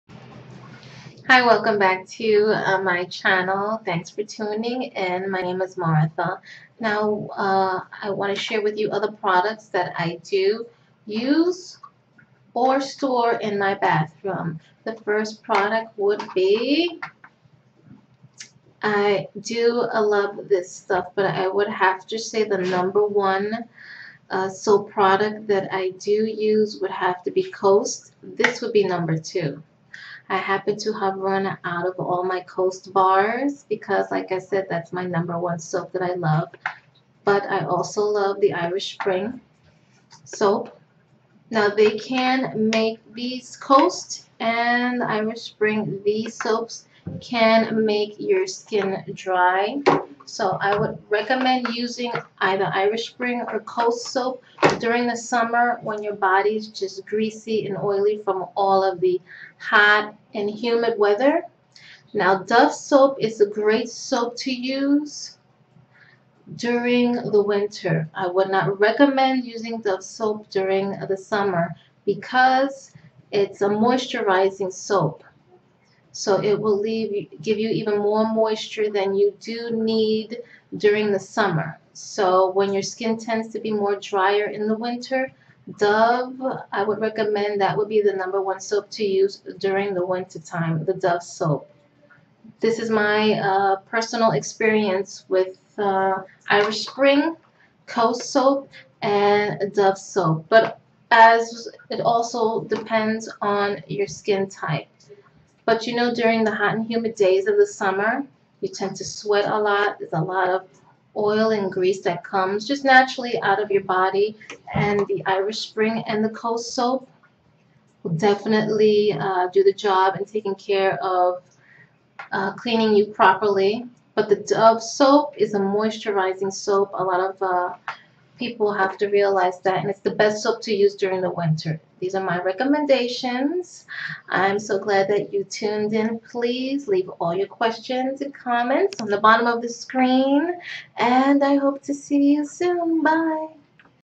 BATHROOM PRODUCTS
COAST SOAP I ADORE MY NUMBER ONE PICK

IRISH SPRING IS MY NUMBER TWO PICK

THEY BOTH SMELL SO GREAT THE SCENT DOES PERK UP YOUR SENSES

DOVE SOAP IS HIGHLY RECOMMENDED FOR THE DRY WINTER MONTHS